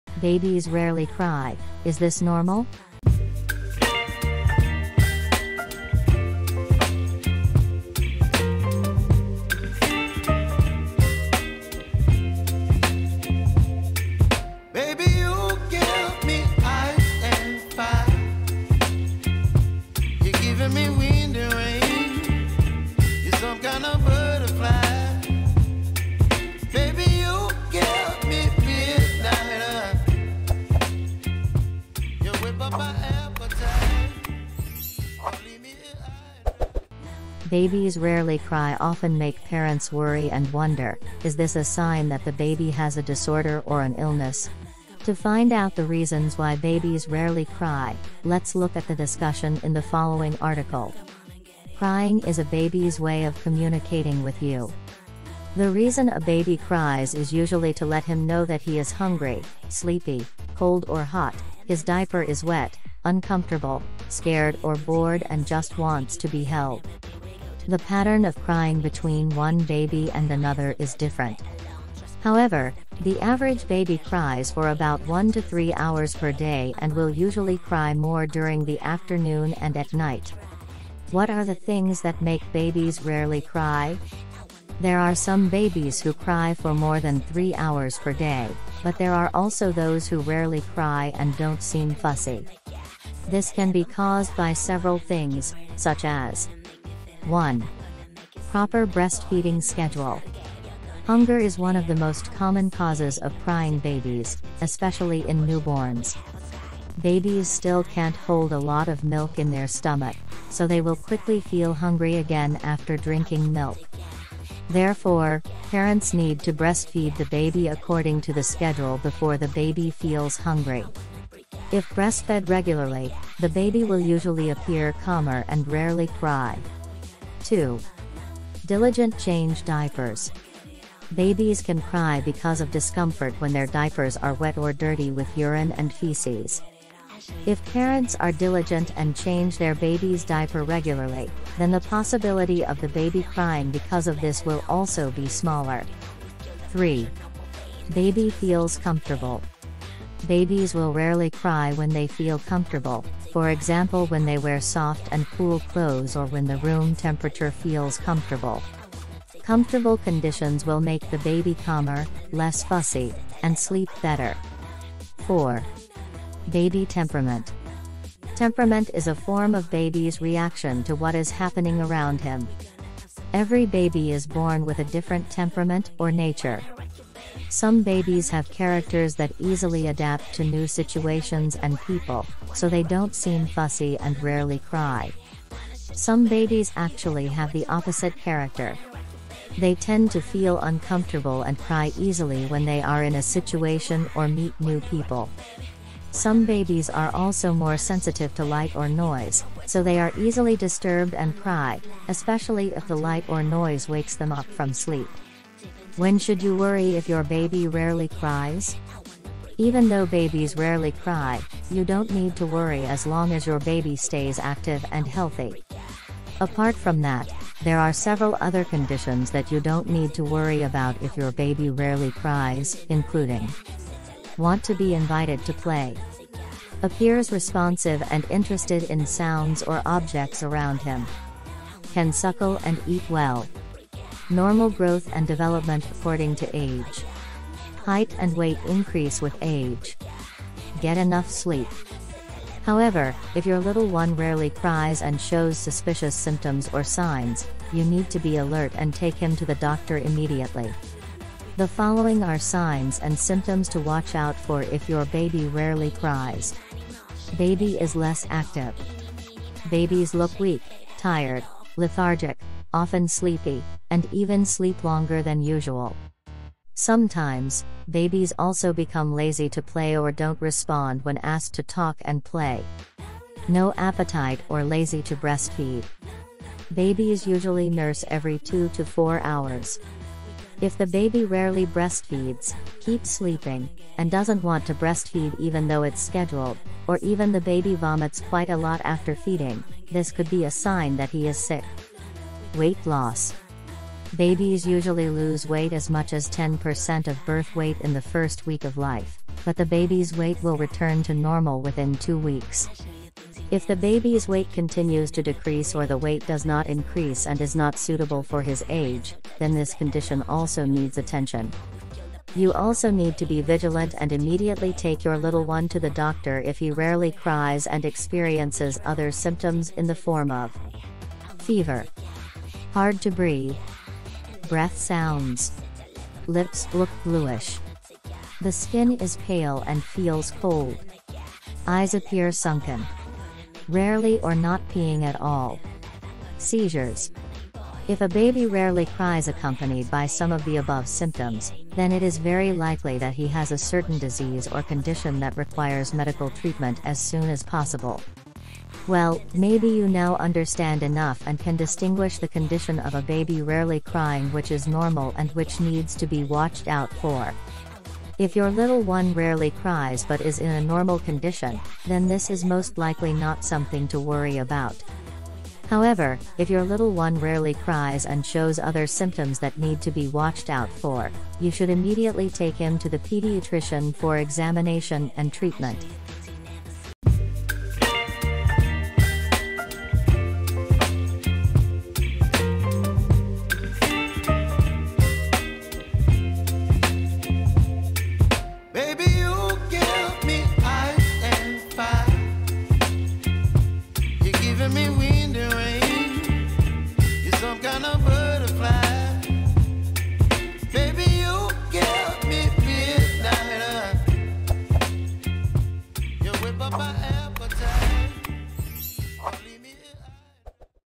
Babies Rarely Cry, Is This Normal
Babies rarely cry often make parents worry and wonder, is this a sign that the baby has a disorder or an illness? To find out the reasons why babies rarely cry, let's look at the discussion in the following article.

Crying is a baby's way of communicating with you. The reason a baby cries is usually to let him know that he is hungry, sleepy, cold or hot, his diaper is wet, uncomfortable, scared or bored and just wants to be held.
XHIT Daily.
Tone It Up.
POPSUGAR Fitness.
DOYOUYOGA.com.